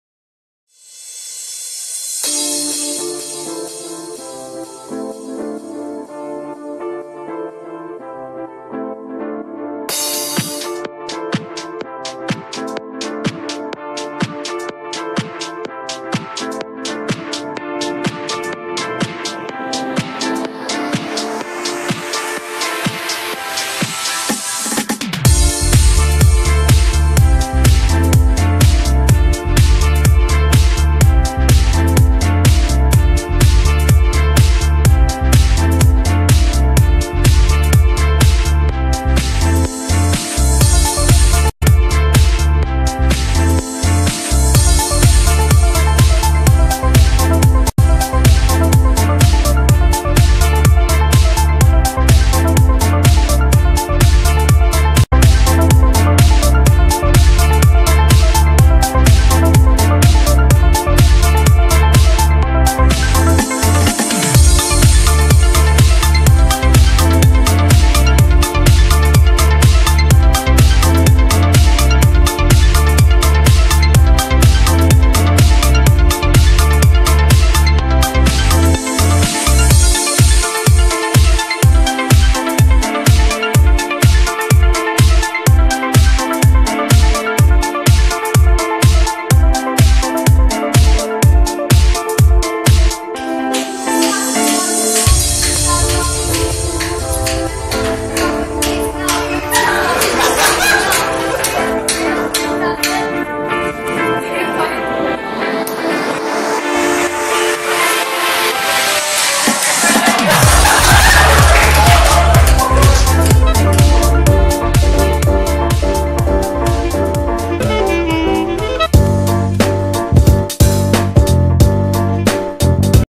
Class fashion week 2017
this is the fashion show performed by the students of class 11